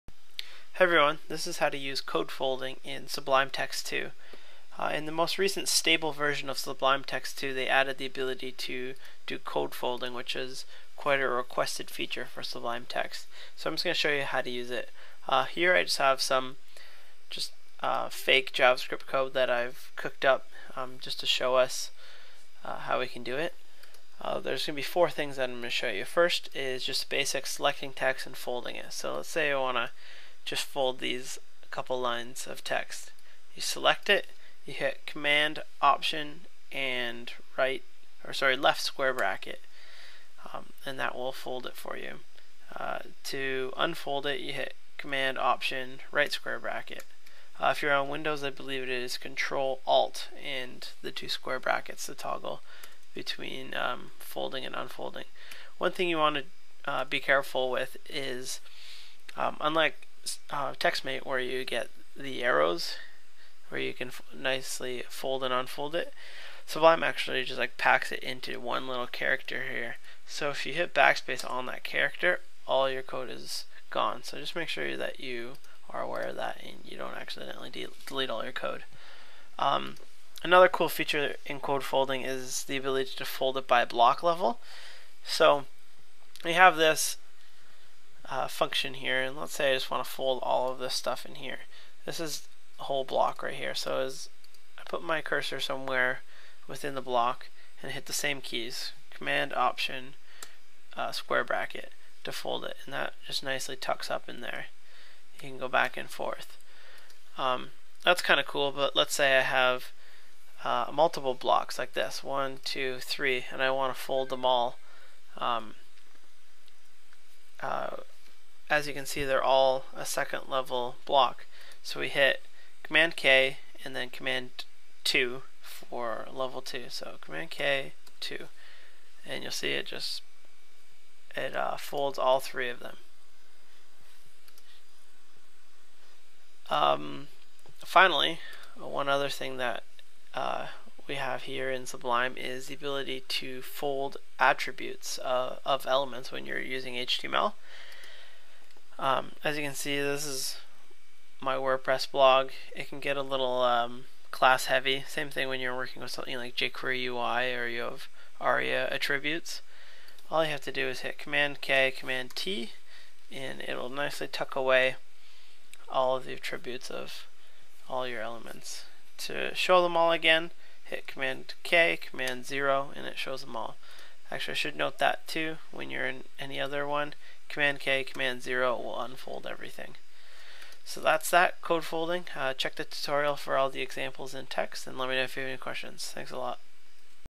Sublime Text Code Folding Tutorial
Sublime Text has just released the code folding functionality in their editor. Here is a quick tutorial on how to use it.